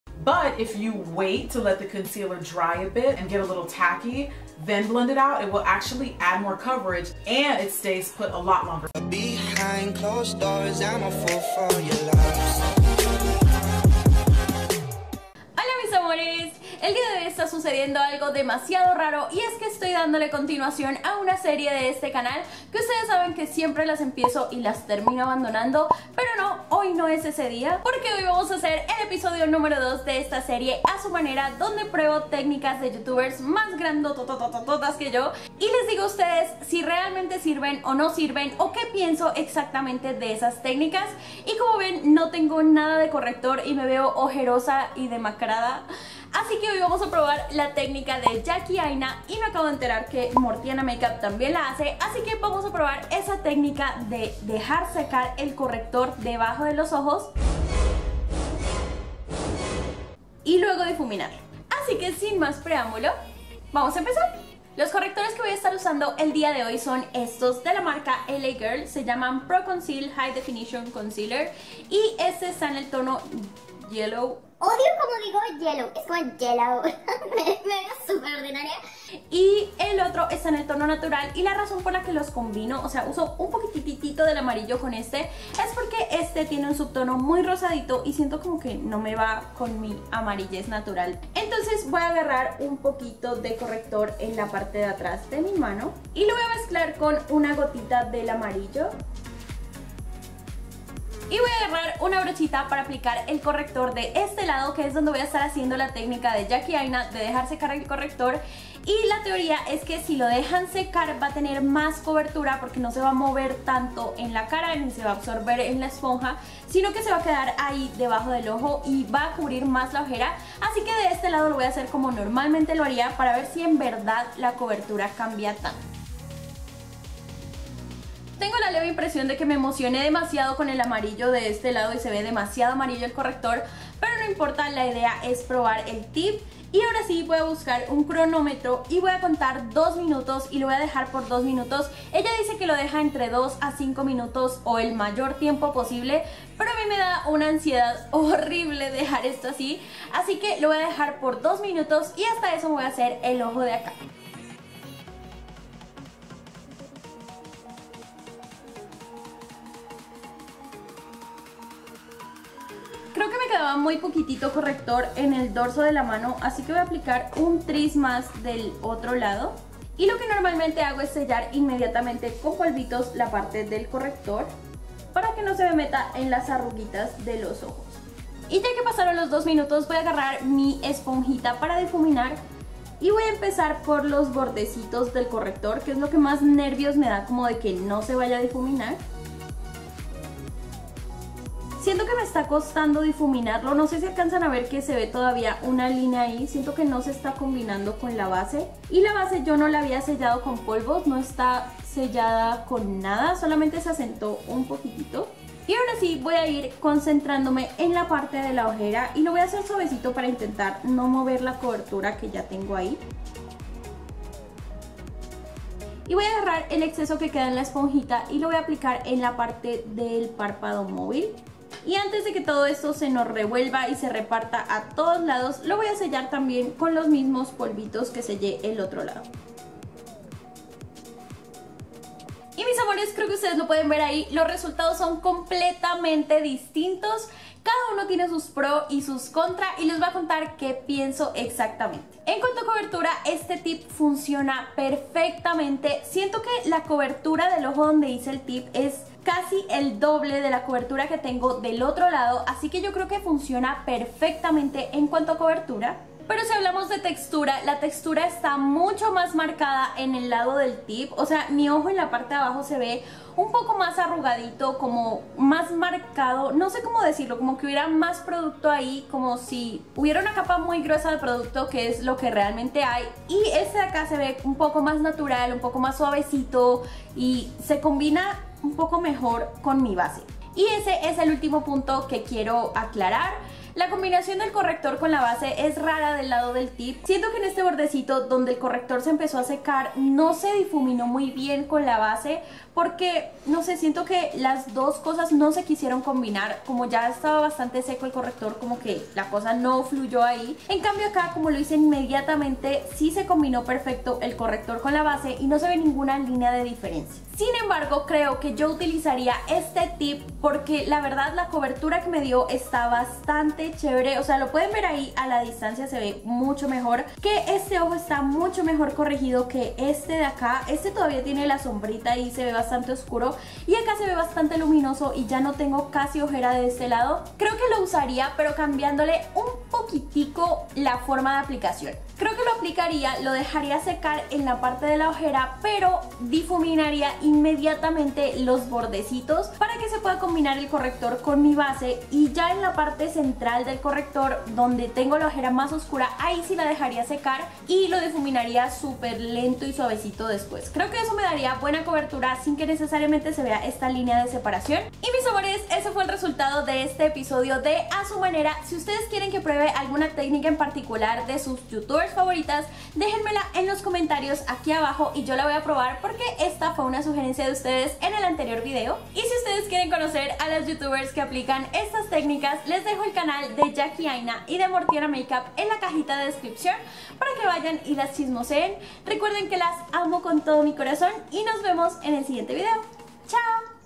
Ojeras totalmente cubiertas? | Tip Jackie Aina | A su manera - Vanessa Bacca
Mis amores! Realmente esta técnica de dejar secar el corrector antes de difuminarlo me dio una ansiedad looooca... Pero ustedes lo pidieron y realmente tenía que probarlo y me quedé en shock!

Corrector:
L.A.Girl Pro Coverage Concealer "Natural" y "Yellow"

Les dejo los links aquí abajito del canal de Jackie Aina y de Mortiana Makeup para que las chismoseen.

 ♥ ♥ ♥ ♥ ♥ ♥ ♥ ♥ SOLO PARA CALI COLOMBIA ♥ ♥ ♥ ♥ ♥ ♥ ♥ ♥

👉🏻 MIRELY MAKEUP

 ♥ ♥ ♥ ♥ ♥ ♥ ♥ ♥ Donde encontrar más  ♥ ♥ ♥ ♥ ♥ ♥ ♥ ♥

 ♥ ♥ ♥ ♥ ♥ ♥ ♥ ♥  Preguntas frecuentes   ♥ ♥ ♥ ♥ ♥ ♥ ♥ ♥